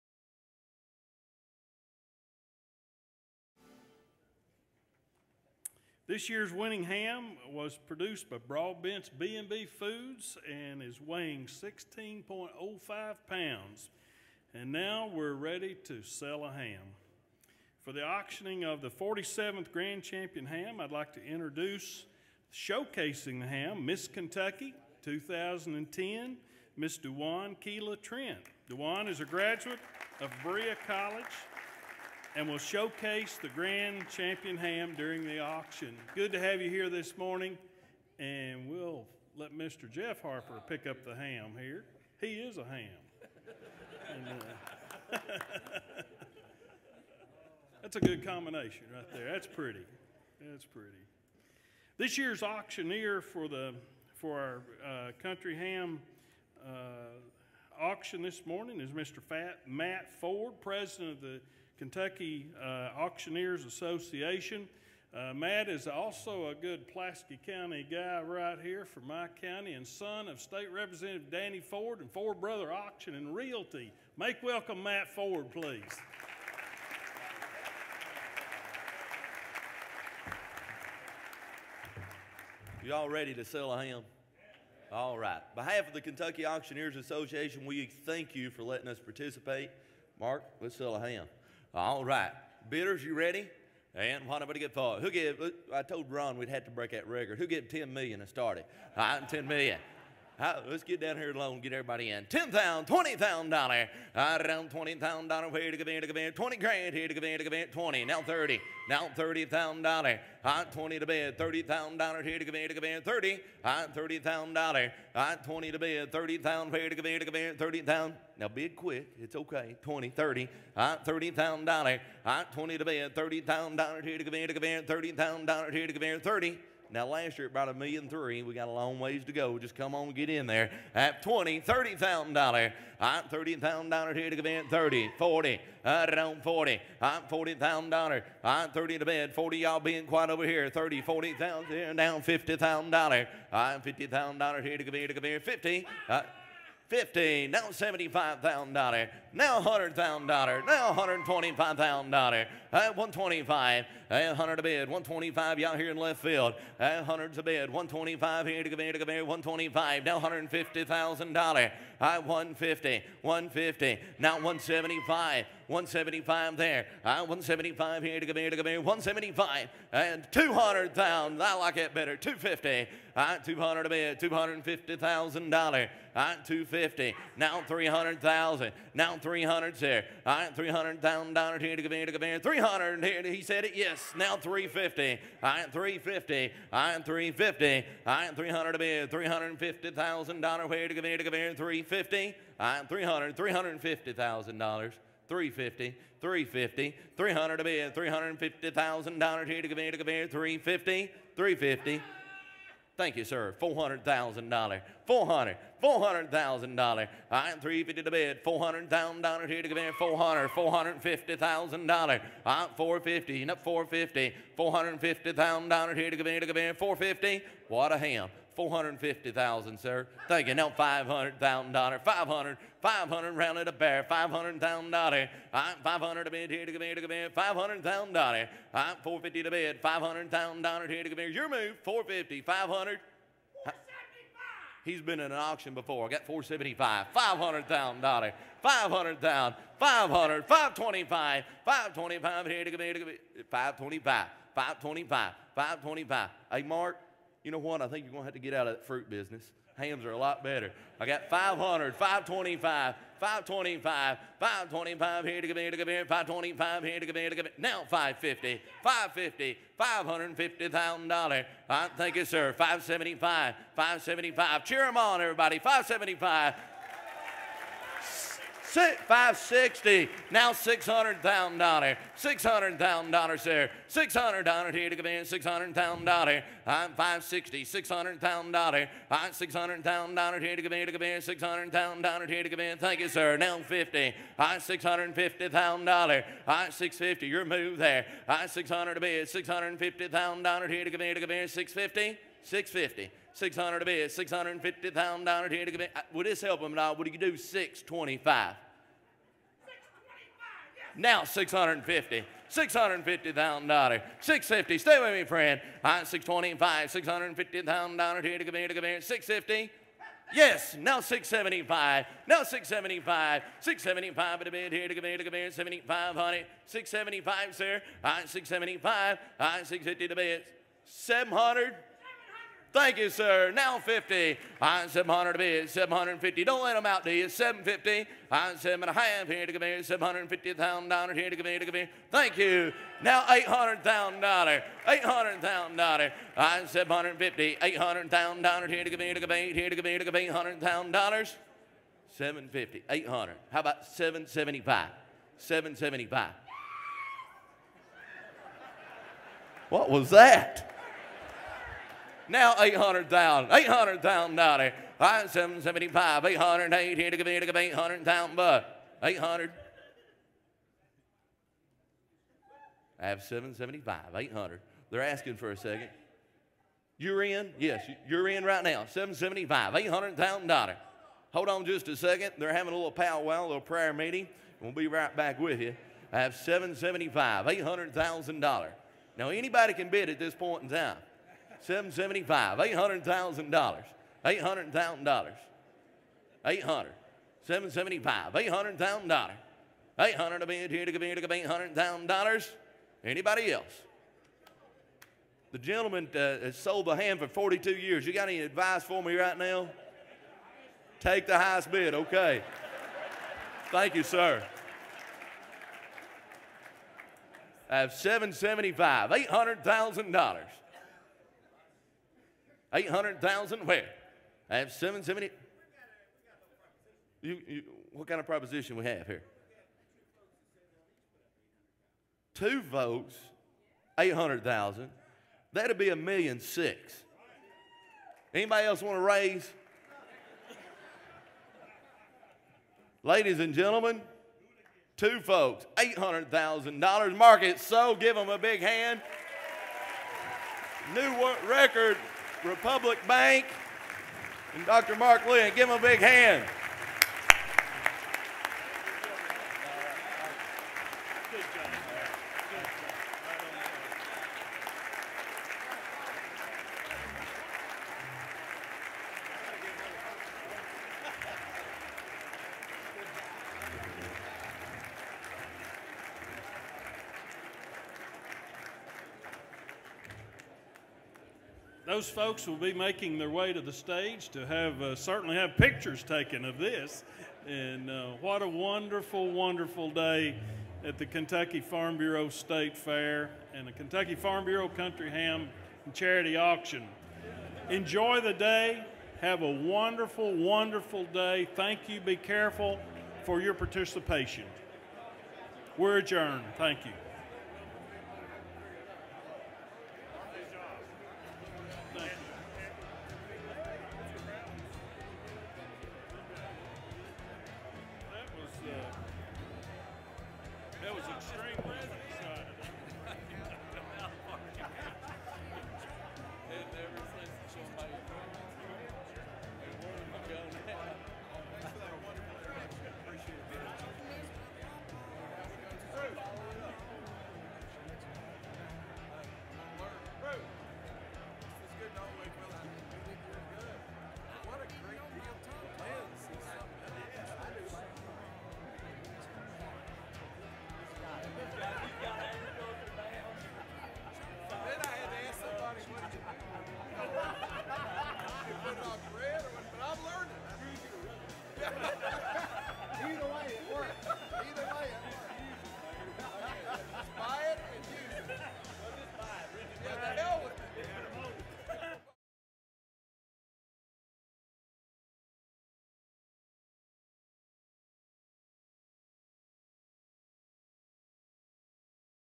Kentucky Farm Bureau 47th Country Ham Breakfast: Ham Auction
Kentucky Farm Bureau's  47th Country Ham Auction raises a record-breaking $1.6 million for charity at the 2010 Kentucky State fair.

Two bidders, Bernard Trager, Chairman of Republic Bank and Trust, and Dr. Mark Lynn & Associates, owner of Dr. Bizer's Vision World, pooled their resources together and contributed $800,000 each for the 16.05-pound ham produced by Broadbent B&B Foods of Kuttawa, Kentucky. That is the equivalent of $100,000 per pound of ham.